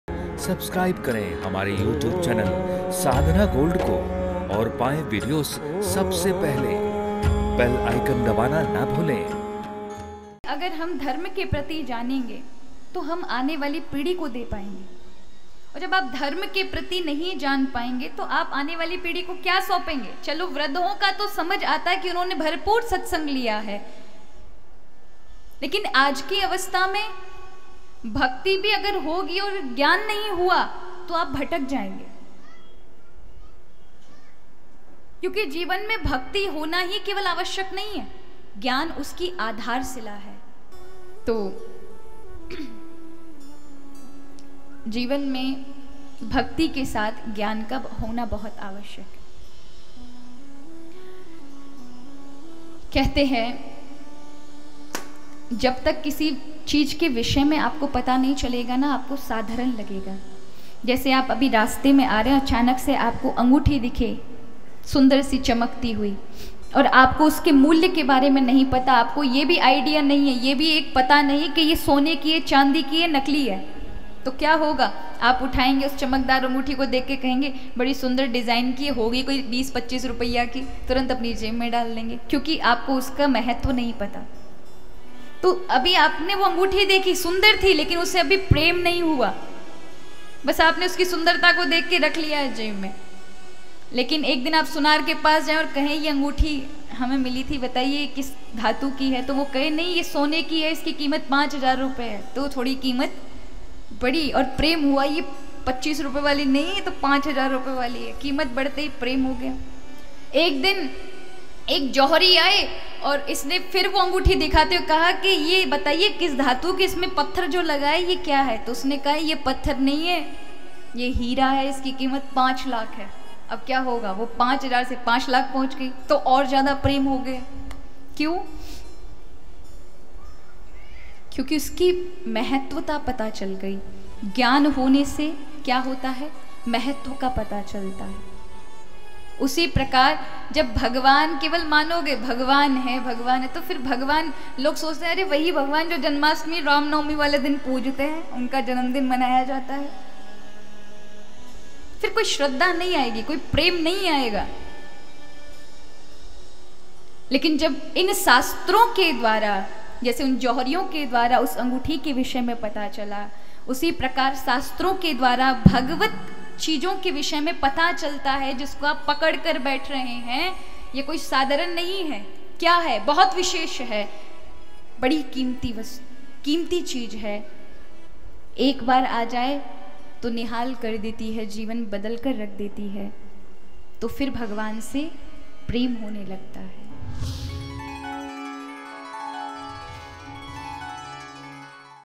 जीवन में भक्ति और ज्ञान का होना क्यों जरुरी है ? पूज्या श्री कृष्णप्रिया जी महाराज।। Sadhna TV
Thanks.